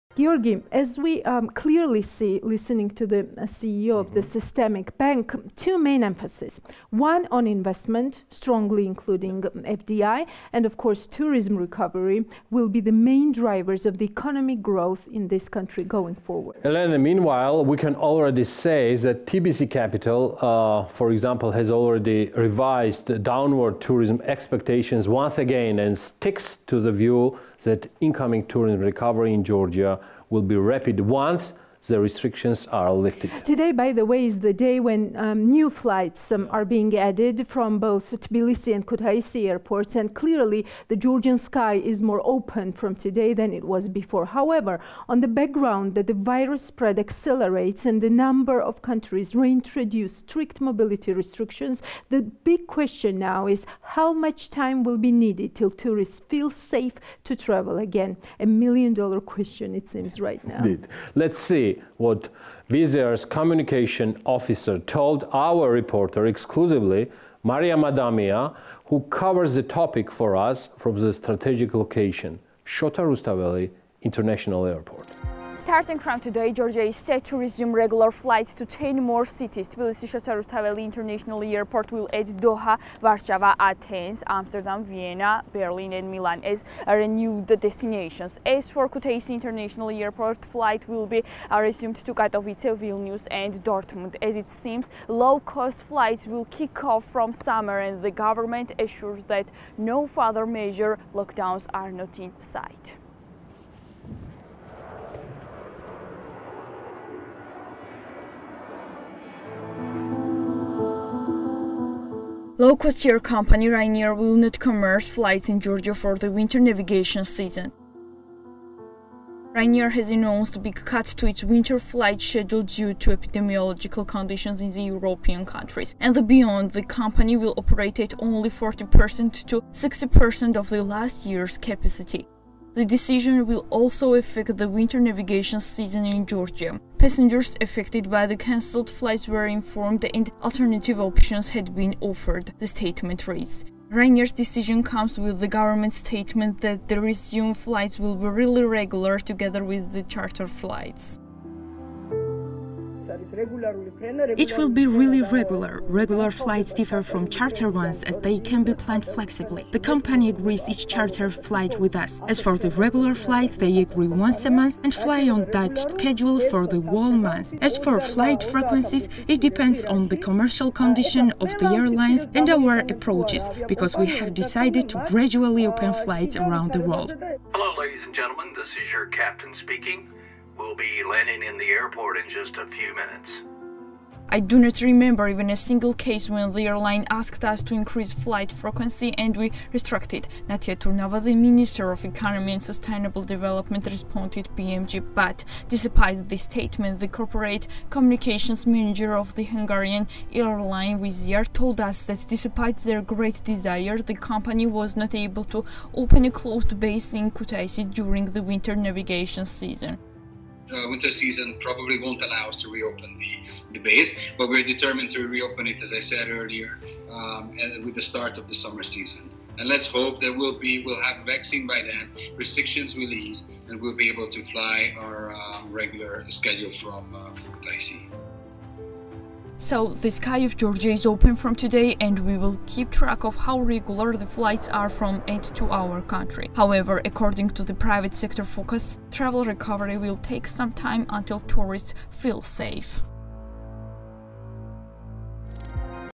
Q&A: Wizziar
Today is the day when new flights are being added from both Tbilisi and Kutaisi Airports and clearly, the Georgian sky is more open from today than it was before. However, on the background that the virus spread accelerates and the number of countries reintroduce strict mobility restrictions, the big question now is how much time will be needed till tourists feel safe to travel again - a million dollar question, it seems right now.